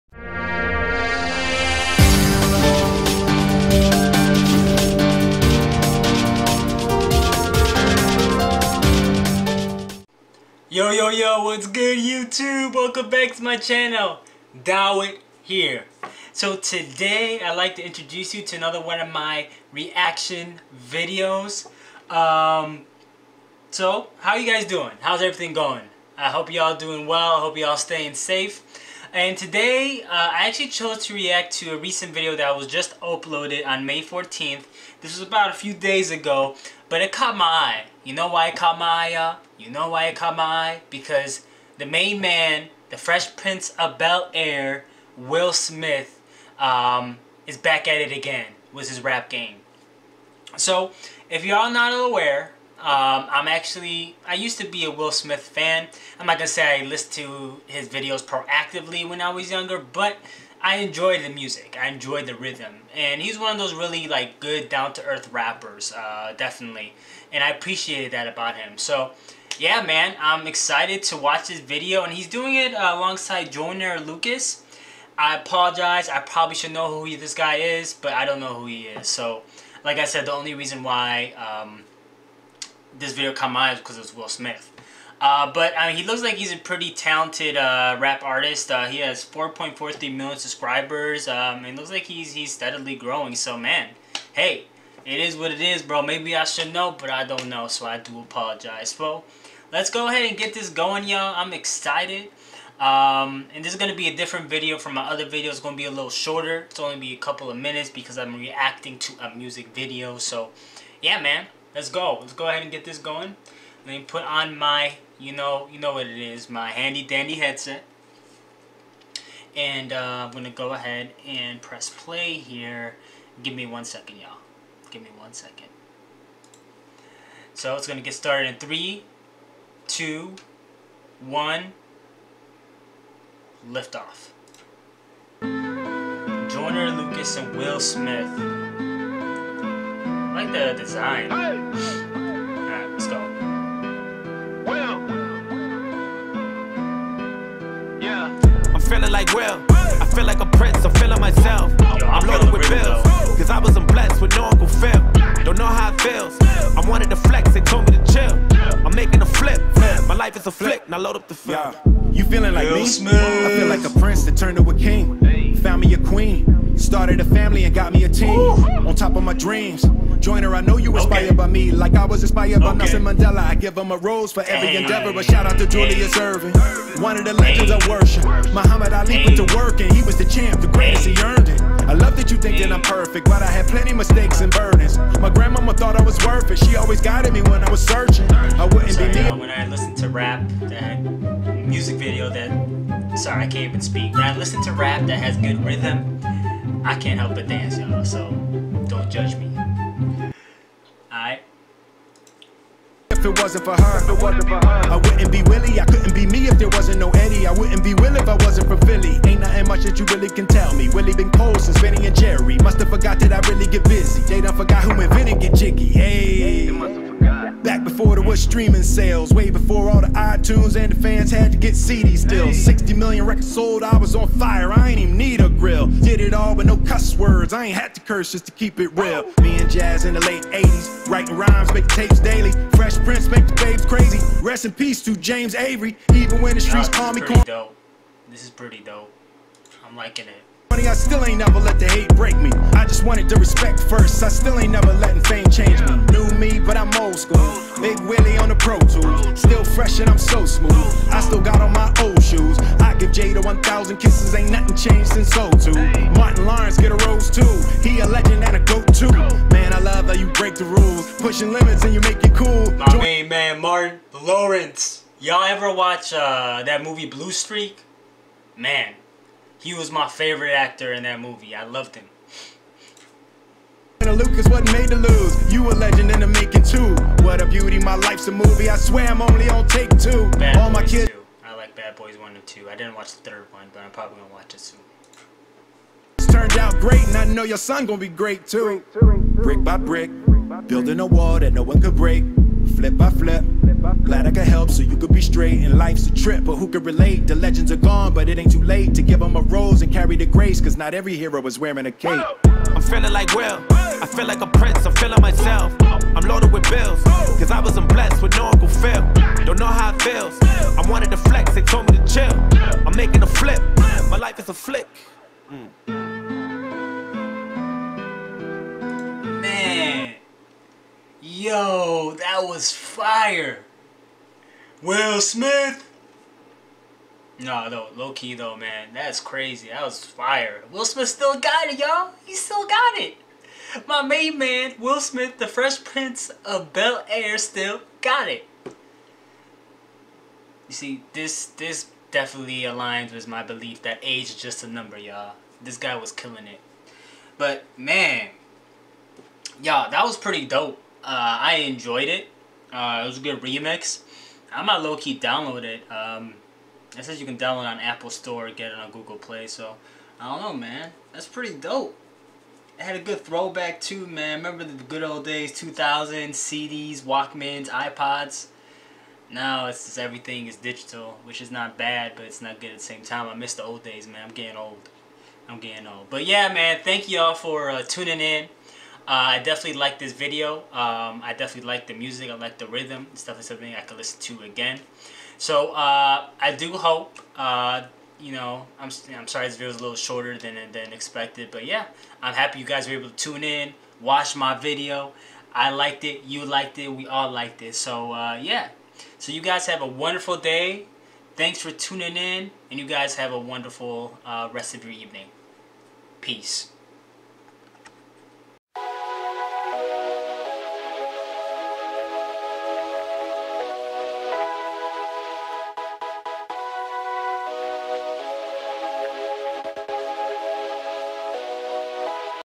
Joyner Lucas & Will Smith REACTION!! Video
New Hit music video hitting the trends!!! Watch me react to the man, the myth, the legend Will Smith!!